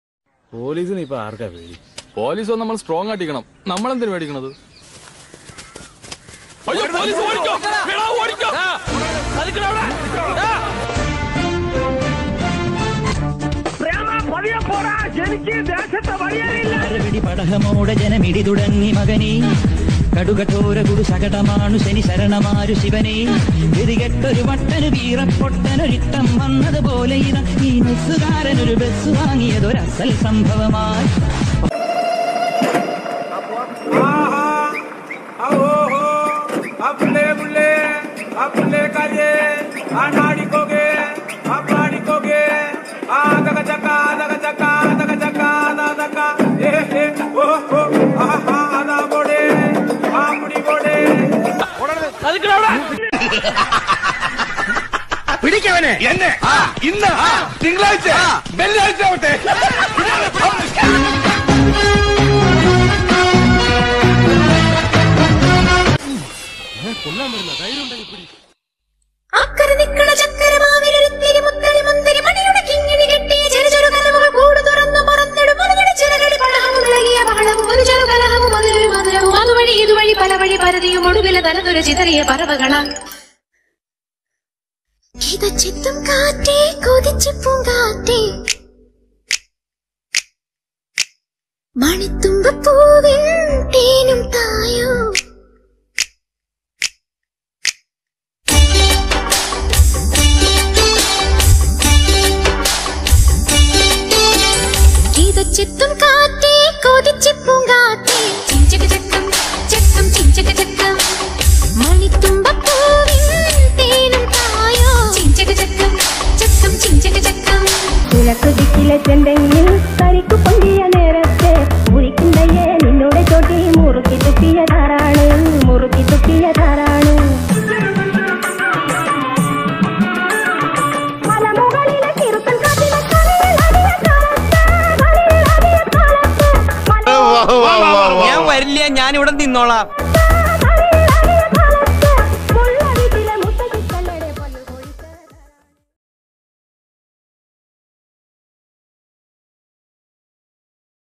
keralapolice_drone കേരളാ പോലീസിന്റെ ചില ഡ്രോൺ കാഴ്ചകൾ.. Funny Visuals By Kerala Police..😁😁😁🤣🤣
കേരളാ പോലീസിന്റെ ചില ഡ്രോൺ കാഴ്ചകൾ..
Funny Visuals By Kerala Police..  സിരിച്ച് ചത്തു😁😁😁🤣🤣

Kerala Police' Drone Patrolling during Lock down